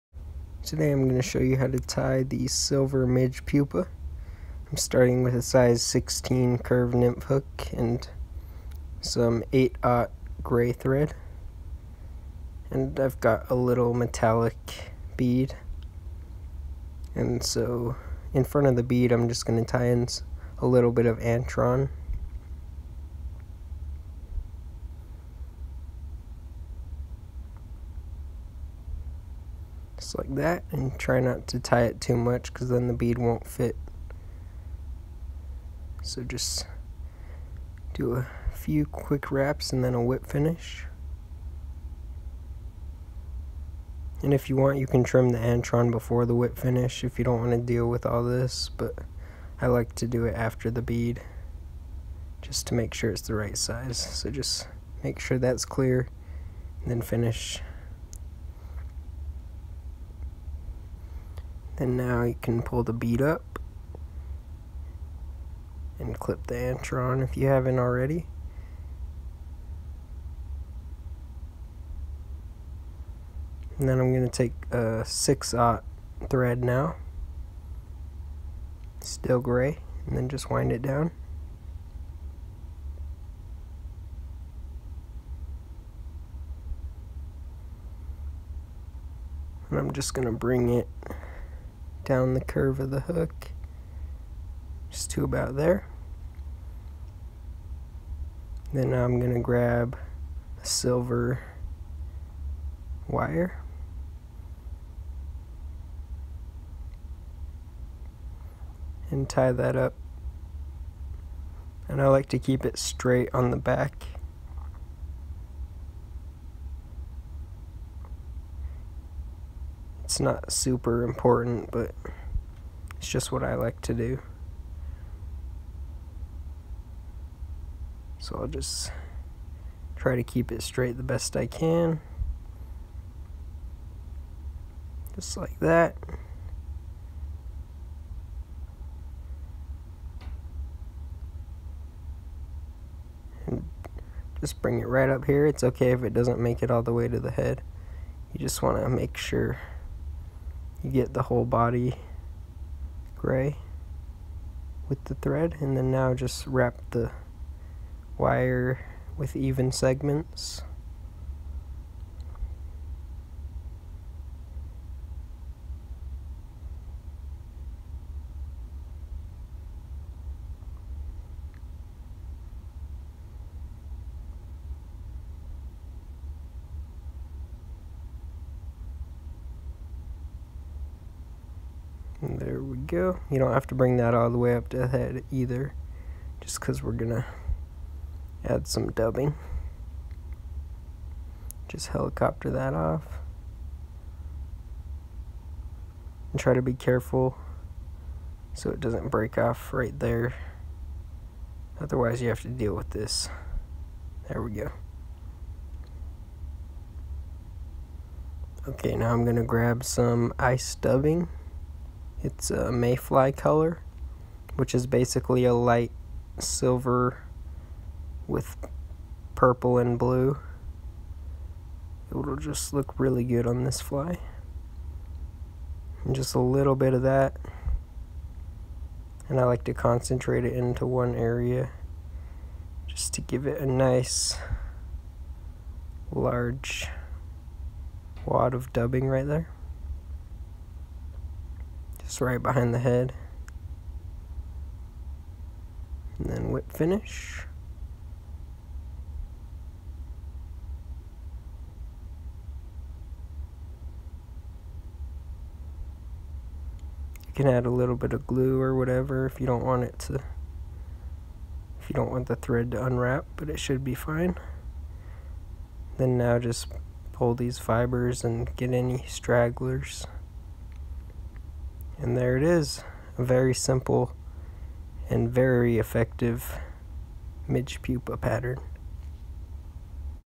Tying The Silver Midge Pupa
Hook: Curved Nymph #16
Bead: Metallic 2mm
Thread: 8/0 and 6/0 Gray
Antenna: Antron White
Body: 6/0 Thread Gray
Ribs: Silver Wire
Thorax: Ice Dubbing Mayfly